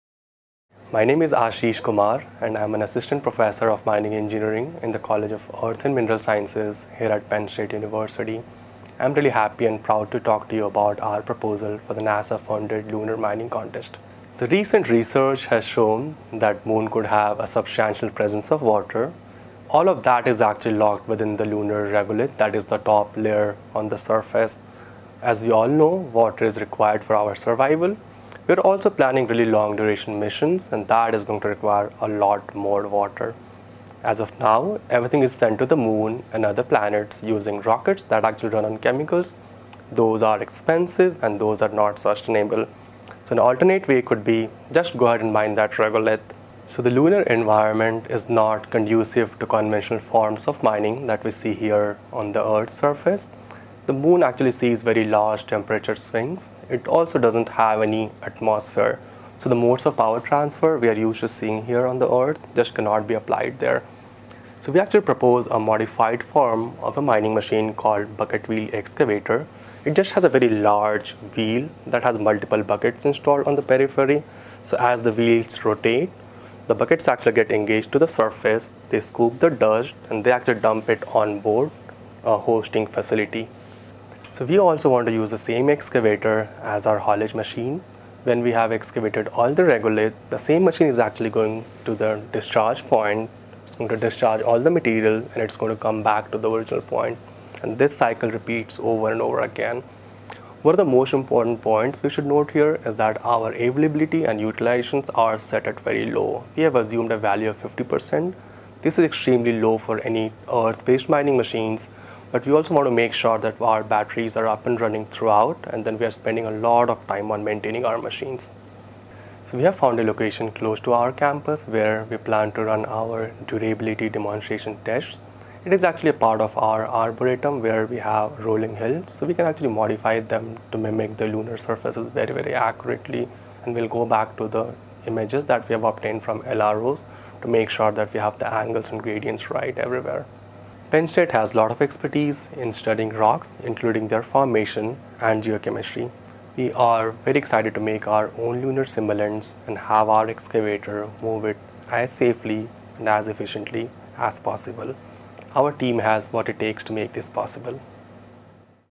Penn State submission to NASA - Break the Ice, lunar mining challenge 2022
The Department of Energy and Mineral Engineering in the College of Earth and Mineral Sciences propose a bucket-wheel excavator variant for lunar regolith mining for the NASA-funded competition. Eight buckets installed in series on a large wheel scoop the regolith and store it temporarily in an onboard chamber. The regolith will be secured within the chamber to alleviate the lunar ice's sublimation into the space's hard vacuum. Low availability and utilization values allow the excavator-transporter to spend a lot of time preparing for operations. Onboard sensors feeding information to computer programs will allow the machine to operate autonomously in the dark region of the lunar south pole where the presence of water was recently established.